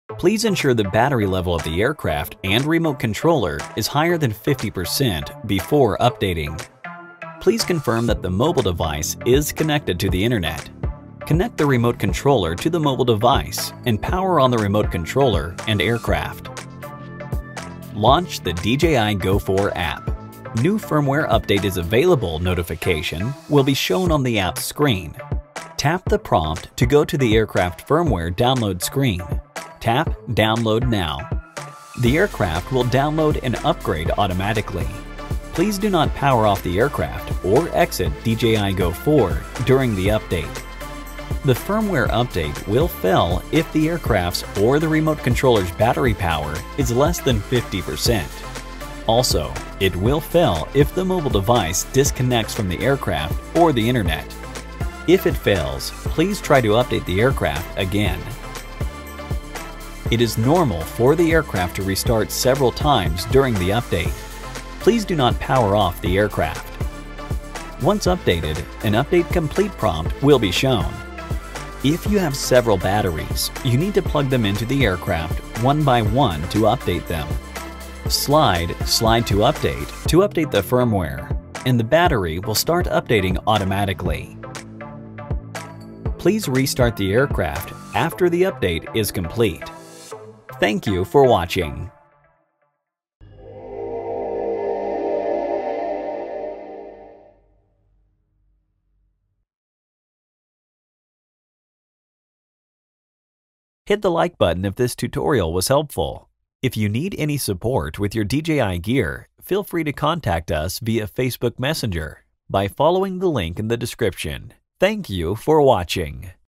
How to Update the Mavic 2 Firmware with DJI GO 4
➤ From now on all new DJI Support videos will be posted on DJI Tutorials channel

This tutorial teaches you how to update Mavic 2 Firmware with DJI GO 4.